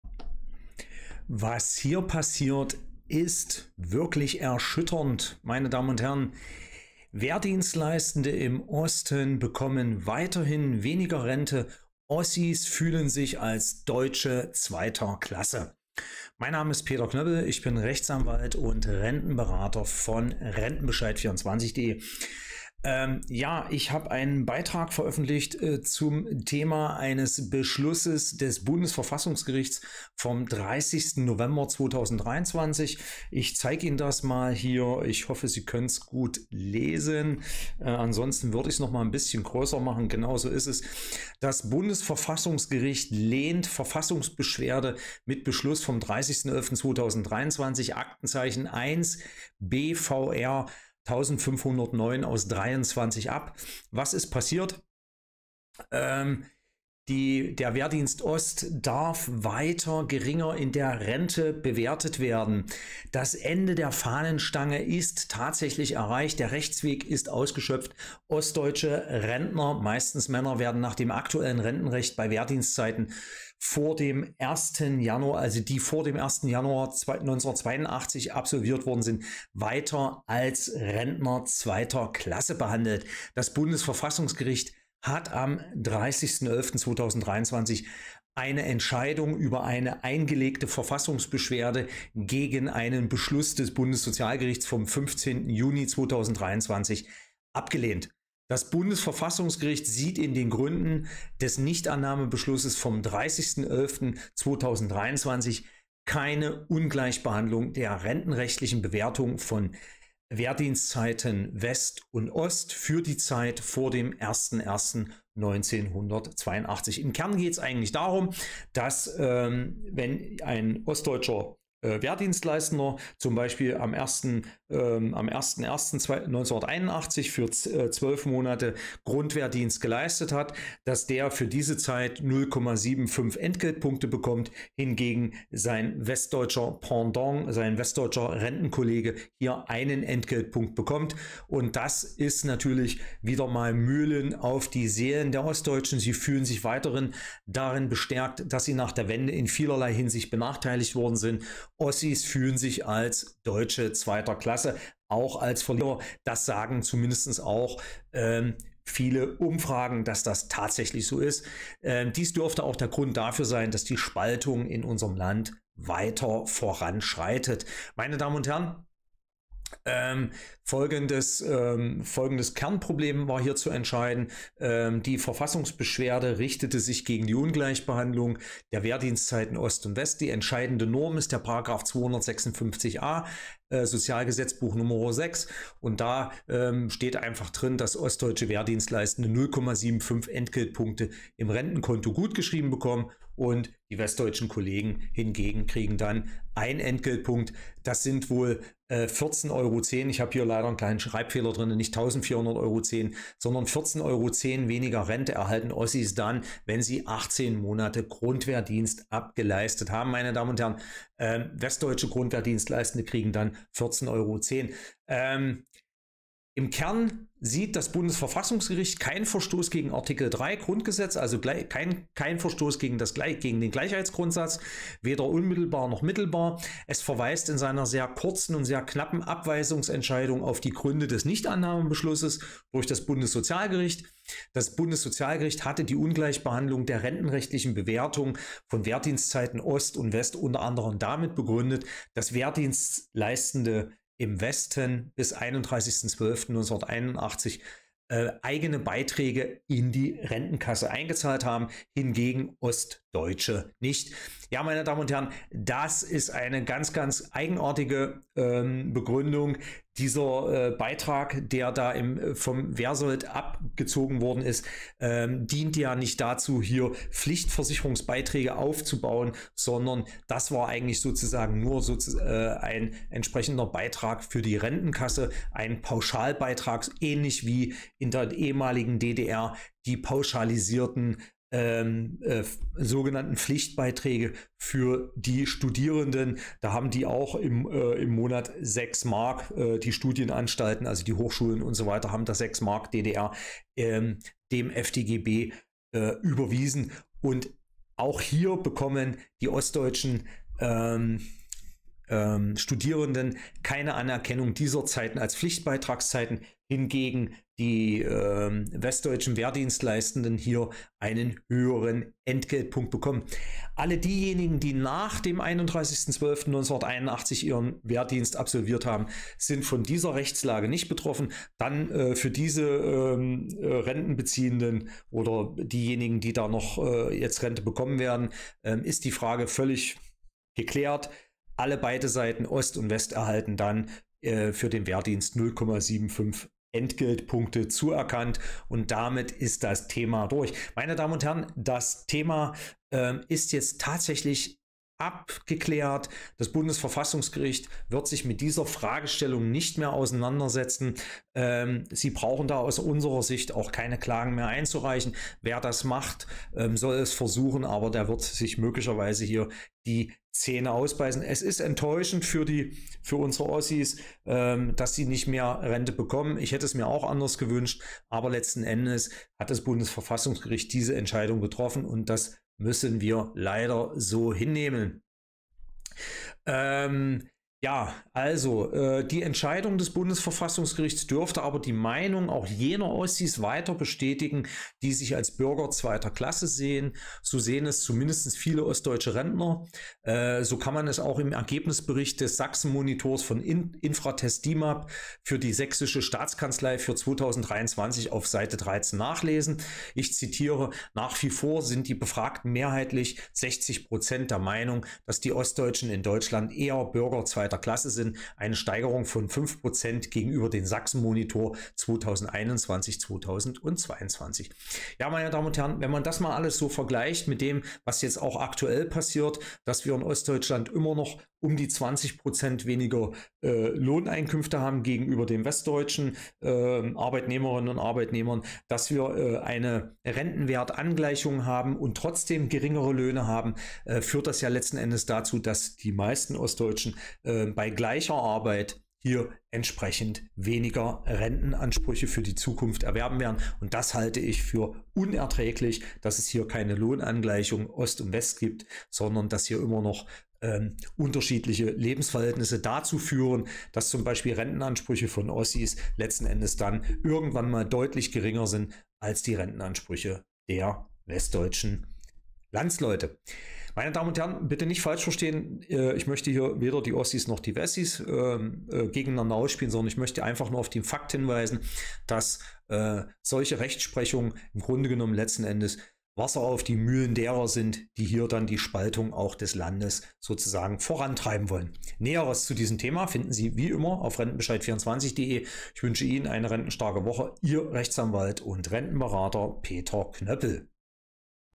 Also doch! Ossis weiter Deutsche zweiter Klasse? Wehrdienstleistende-Ost erhalten weniger Rente
Das Ende der Fahnenstange ist erreicht. Der Rechtsweg ist erschöpft. Ostdeutsche Rentner werden nach dem aktuellen Rentenrecht bei Wehrdienstzeiten vor dem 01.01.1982 weiter als Rentner zweiter Klasse behandelt. Das Bundesverfassungsgericht lehnte am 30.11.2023 eine Entscheidung über eine Verfassungsbeschwerde gegen den Beschluss des Bundes­sozial­gerichtes vom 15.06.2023, Aktenzeichen: B 5 R 67/23 B, ab.
Das Bundesverfassungsgericht hat am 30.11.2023 endgültig über die Rechtsfrage entschieden, ob Wehrdienstzeiten Ost und West bis einschließlich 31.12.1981 unterschiedlich rentenrechtlich bewertet werden dürfen. Das Ergebnis ist für viele hunderttausende Ostdeutsche Rentner enttäuschend. Das BVerfG weist eine Verfassungsbeschwerde mit einem Nichtannahmebeschluss kurz und schmerzlos ab.
Hintergrund des Rechtsstreits: Der Rechtsstreit richtet sich gegen die rentenrechtlich unterschiedliche Bewertung von Wehrdienstzeiten Ost und West. Entscheidende Norm ist der § 256a SGB VI. Wehrdienstleistende aus dem Osten erhalten bis bis einschließlich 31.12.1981 für Wehrdienstzeiten 0,75 Entgeltpunkte im Rentenkonto gutgeschrieben, die Wessis hingegen bekommen für ihren Wehrdienst 1,0 Entgeltpunkte. 1410€ weniger Rente erhalten Ossis wenn sie 18 Monate Grundwehrdienst abgeleistet haben.
Das Bundesverfassungsgericht sieht in den Gründen des Nichtannahme­beschlusses vom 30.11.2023 keine Ungleich­behandlung der rentenrechtlichen Bewertung von Wehrdienstzeiten West und Ost für die Zeit vor dem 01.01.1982. Der Beschluss des BVerfG vom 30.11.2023 ist Wasser auf die Mühlen der Seelen der Ostdeutschen. Sie fühlen sich weiter bestärkt darin, dass sie nach der Wende in vielerlei Hinsicht benachteiligt wurden. Ossis fühlen sich als Deutsche zweiter Klasse, als Verlierer. Dies dürfte auch der Grund dafür sein, dass die Spaltung in unserem Lande weiter vorangeht.